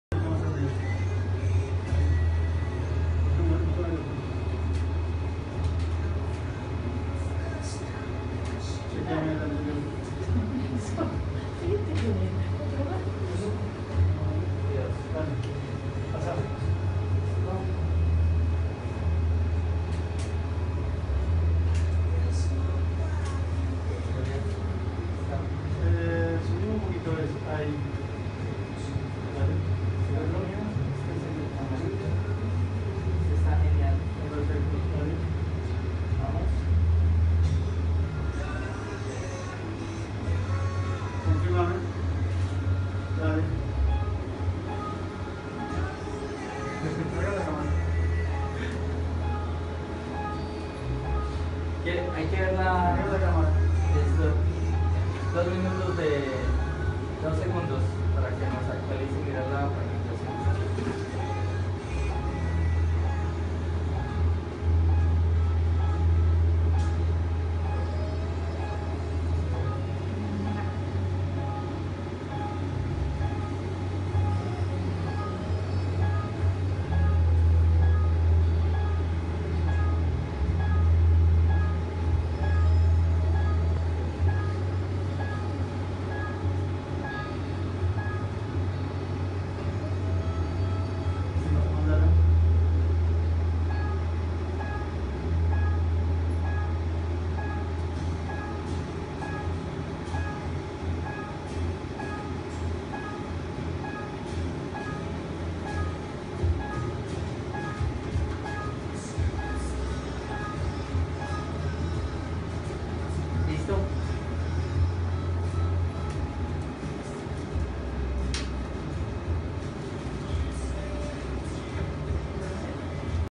Cirugía Femto catarata con LDV Z8, Dr. Ariel Smurra
Una intervención quirúrgica a cargo del Dr. Smurra del instituto Good Vision de Buenos Aires